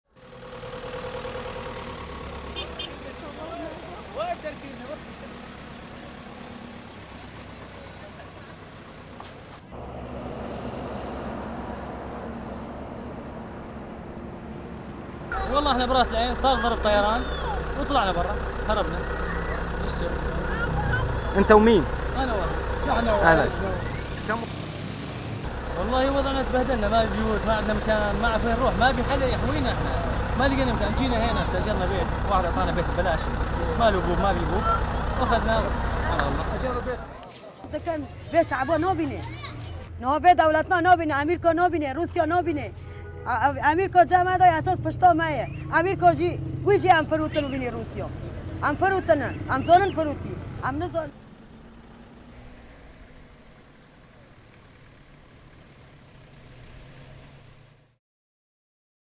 In Syria, history repeats itself for displaced Kurds | AFP
Syrian-Kurds flee the northern border town of Ras al-Ain and take shelter at a school in Hassakeh as Turkey continues their invasion to seize key border towns, on the third day of a broad offensive that sparked a civilian exodus.